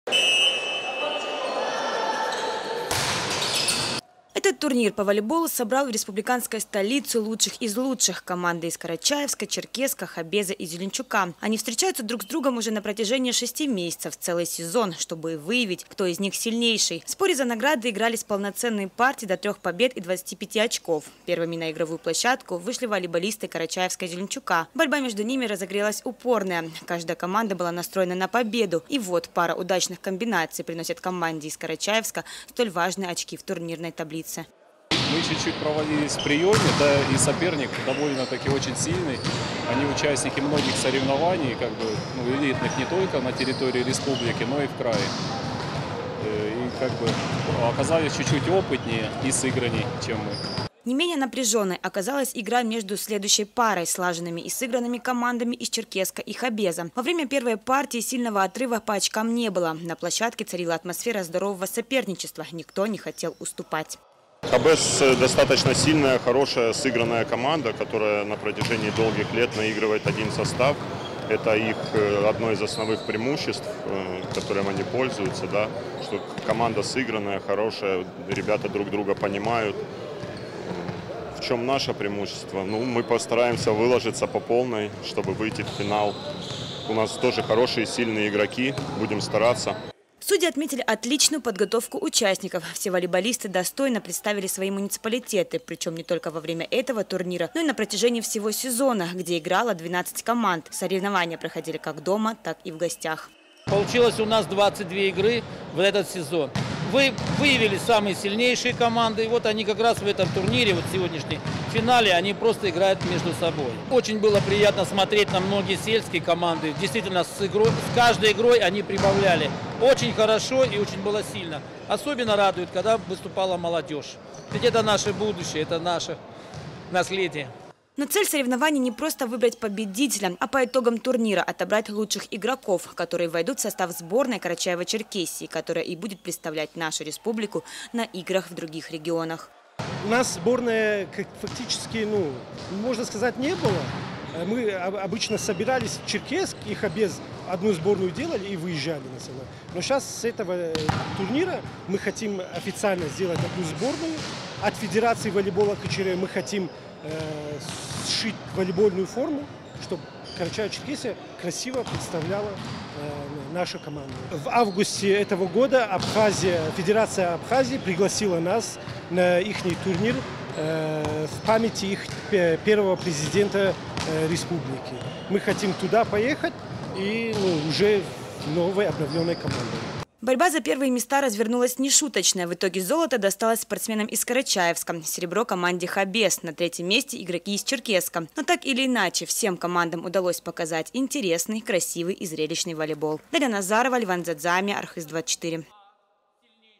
В Черкесске определились победители финальных матчей волейбольной лиги КЧР
Накануне в Черкесске прошли последние, финальные матчи волейбольной лиги КЧР. О накале спортивной борьбы и распределении призовых мест  в нашем сюжете.